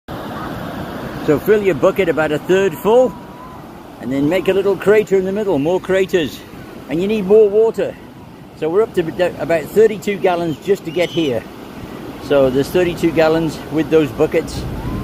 filling the bucket..about 1/3 full, make a hole in its center
Andy Hancock Shows families almost every day how to build massive sandcastles on the Best beach in Texas  @visitspi  (southpadreisland)
This castle weighs 1600lbs used 60 gallons of water to build it and took 5 hours
Social media links to see and learn to build Great

Follow my channels about Building a ContainerHome in Texas